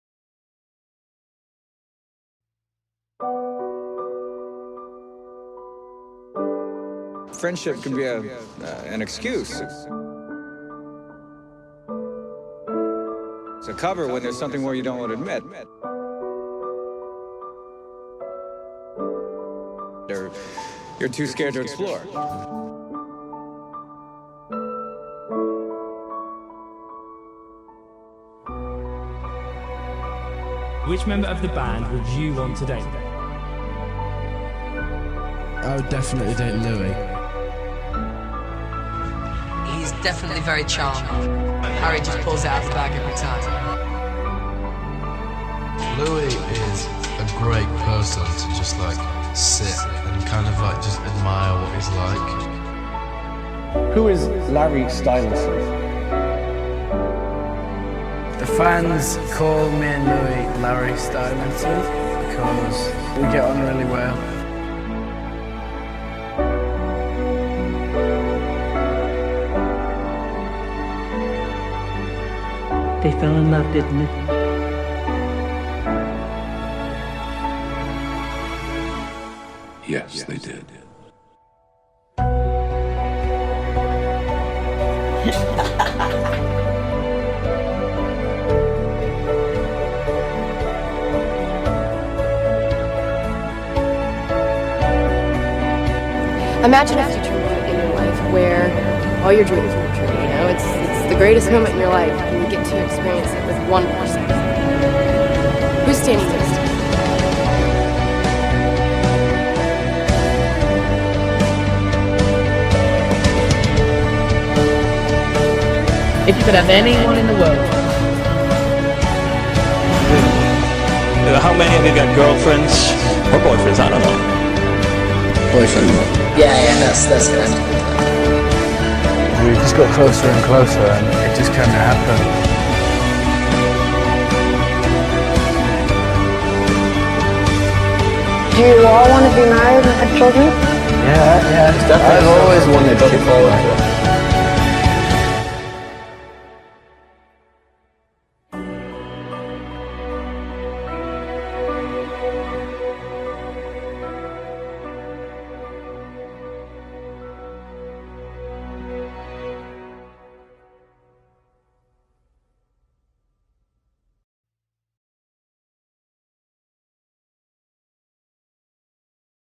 Harry&Louis #15 | "¿Se enamoraron,verdad?" por GossipSmile || SUBTITULADO EN ESPAÑOL
WOOHO,al fin subtitule un video super pedido jajaja :) Hoy voy a intentar de subtitular mas videos de Larry, youtubers , 1D o the vamps.

LEER: queria hacer un video crack de Larry pero en español ¿que piesan? :)